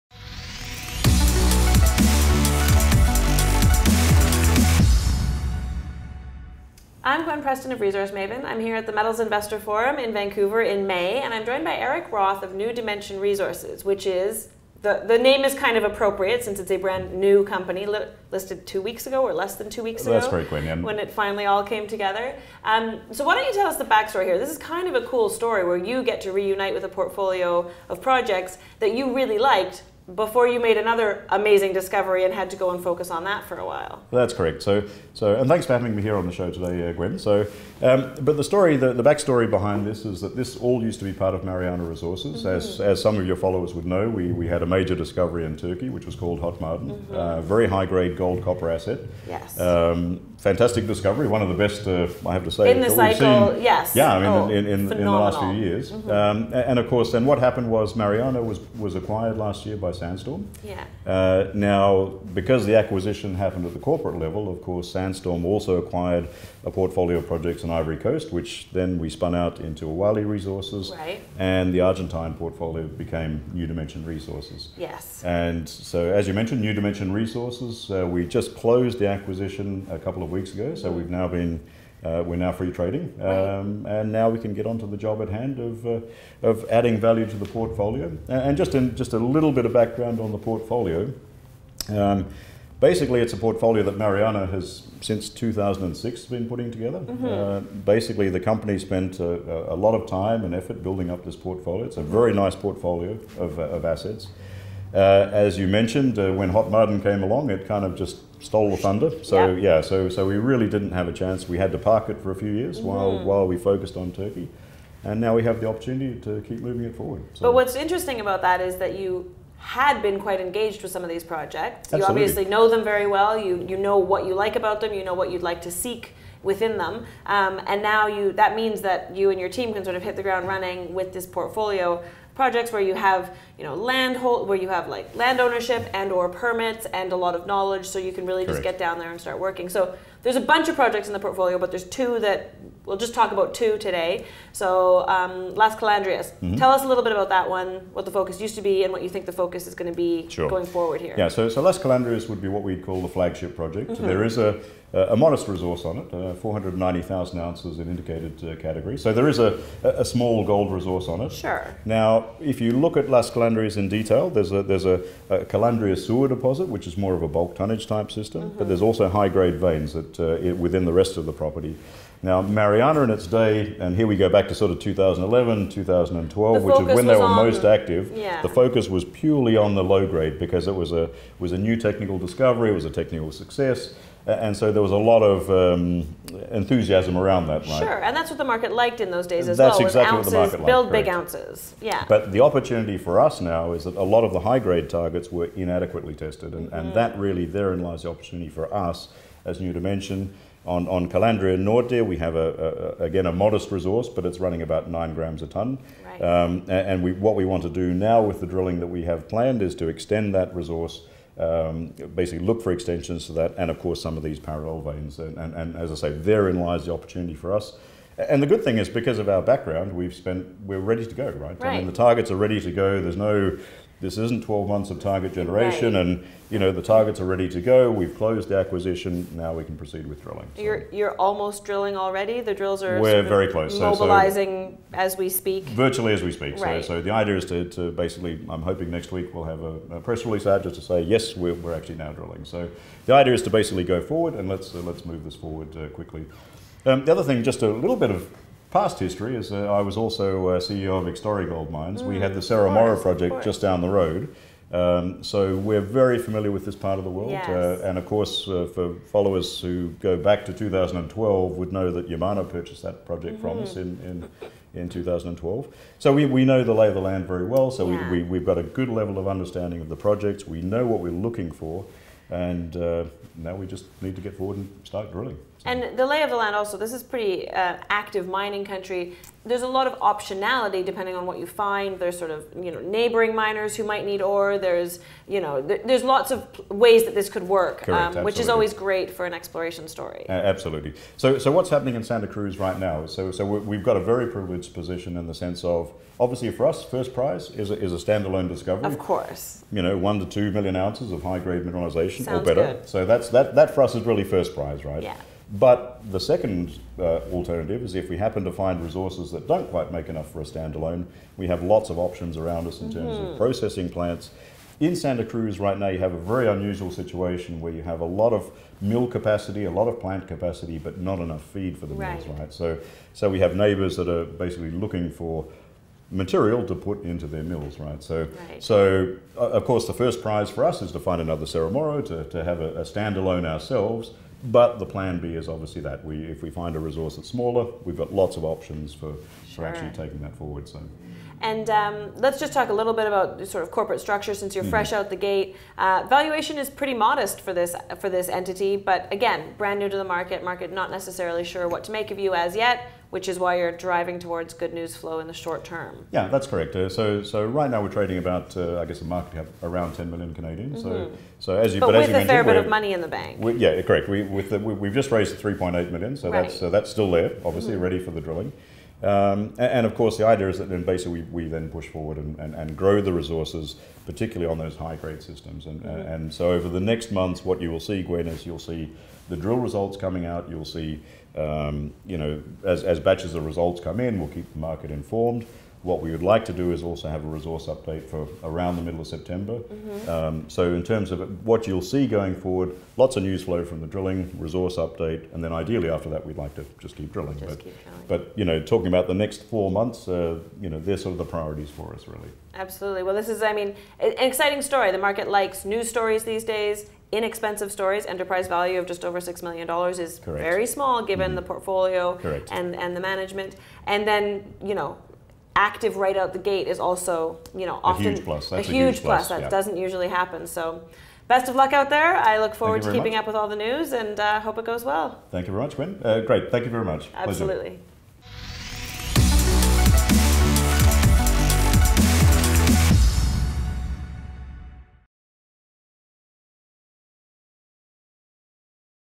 "Las Calandrias Flagship Project" Gwen Preston and Eric Roth of New Dimension Resources Ltd.
Gwen Preston, Resource Maven and Eric Roth, President, CEO and Director of New Dimension Resources Ltd. sit down at the May 2018 Metals Investor Forum in Vancouver.

Don’t forget to sign up for Metals Investor Forum’s Video Library